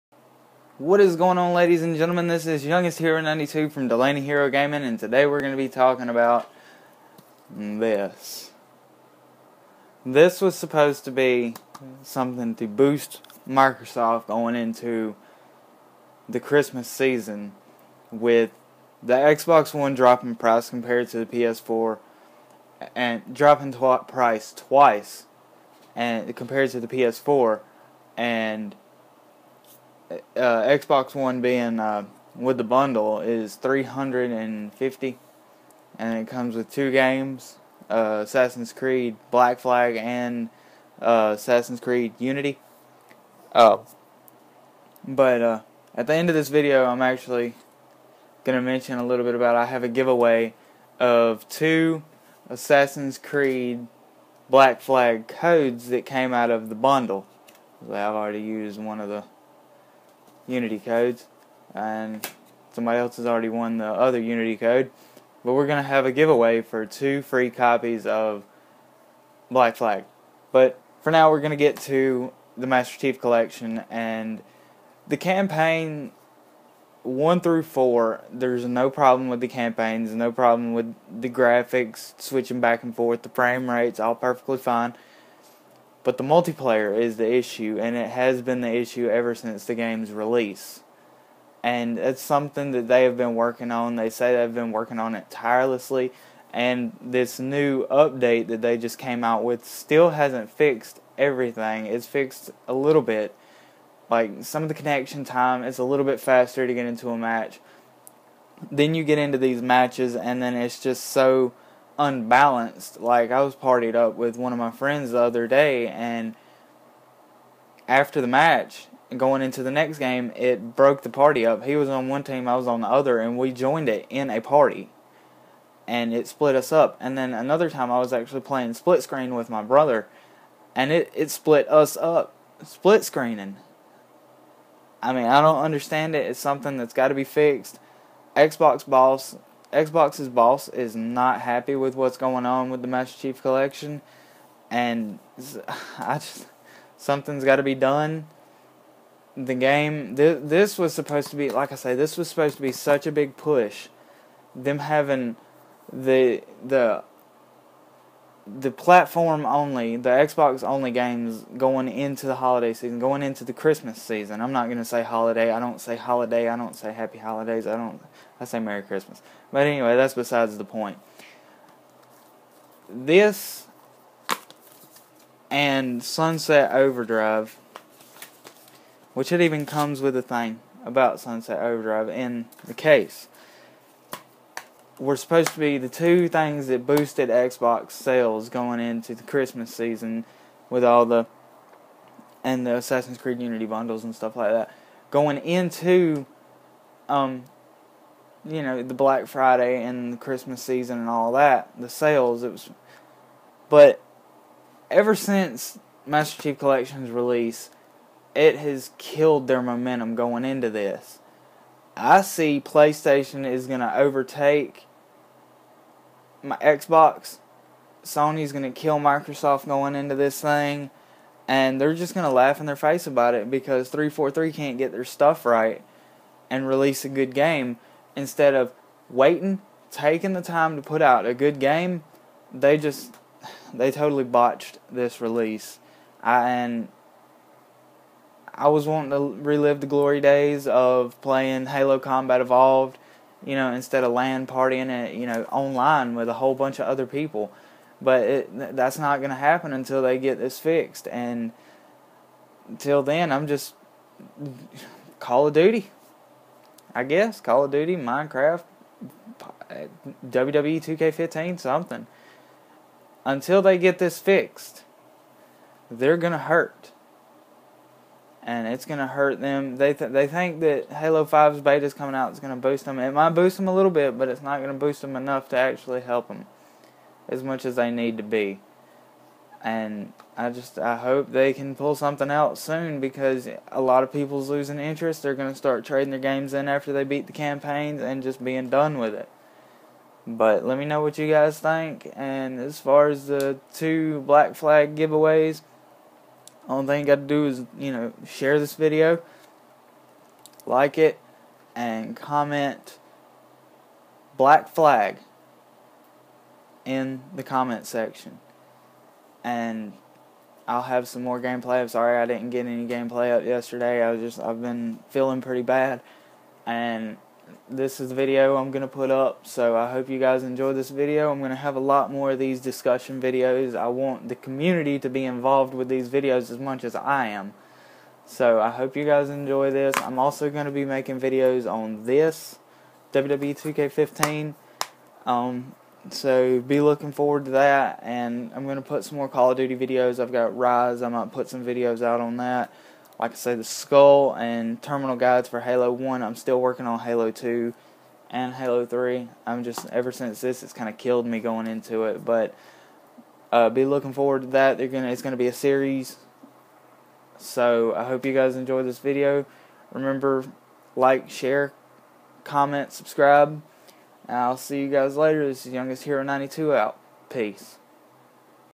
My thoughts on HALO the master chief collection so far +black flag giveaway!!
What's up guys hope you enjoy this discussion video about halo and its multiplayer so far after the update. Have a good day!